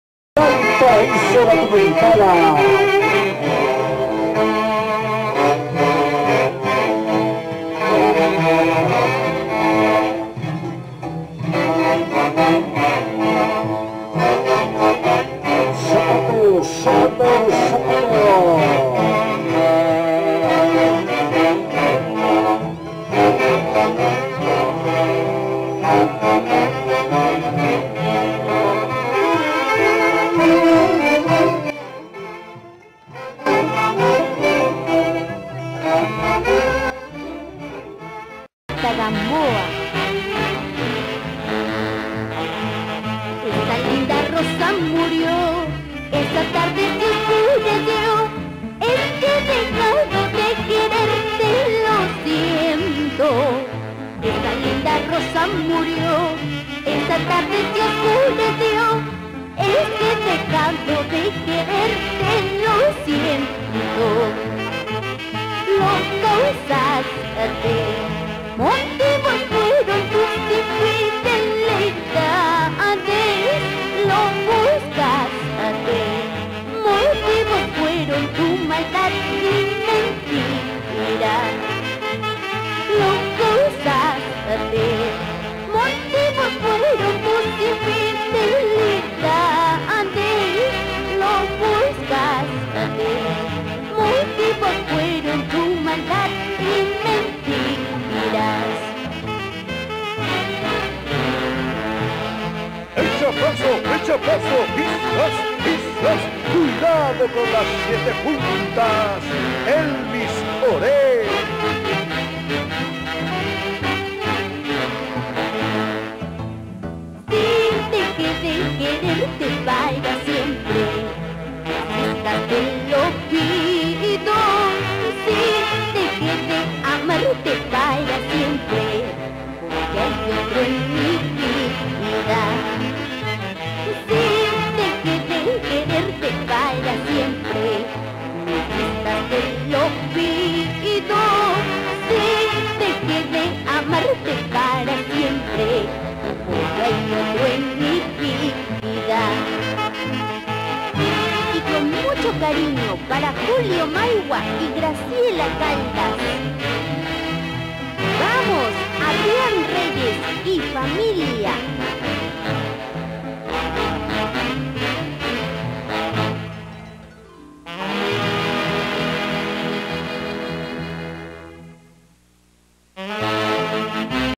HUANCAYO/YAUYOS SU FOLKLOR Y MUSICA
-DANSAS DE LA REGION DEL CENTRO DEL PERU Y SU MUSICA COMO ES LA CHONGINADA Y OTROS QUE REPRESENTAN AL CENTRO DEL PERU